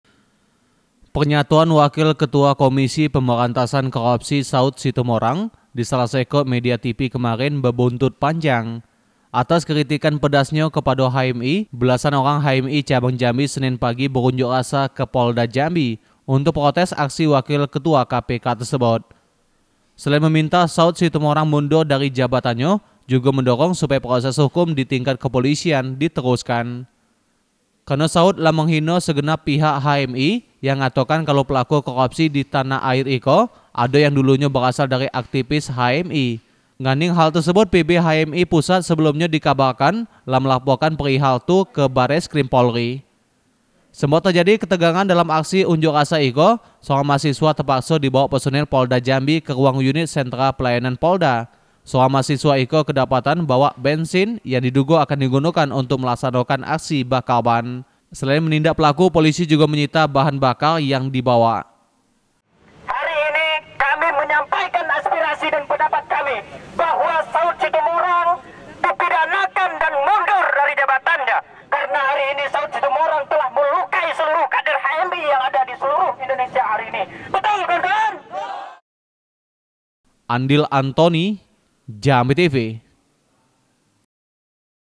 Unjuk Rasa HMI Protes Penyataan Wakil Ketua KPK
Puluhan aktifis Himpunan Mahasiswa Islam Cabang Jambi, Senin Pagi berunjuk rasa di Polda Jambi. Dalam misi protes pernyatoan Wakil Ketuo KPK Saut Situmorang yang dinilai lah mencedarai HMI.

Facebook.com: Jambi-TV
Youtube.com: JambiTV Kebangaan Kito